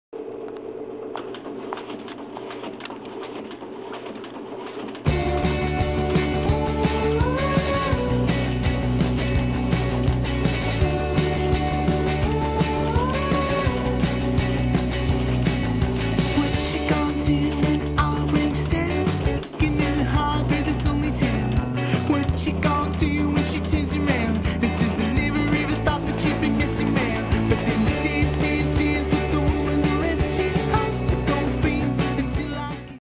Morgana FRN 5
Demonstration of a Morgana friction fed numbering & perforating machine, model FRN 5. Shows the simple operation and paper feeding capabilities.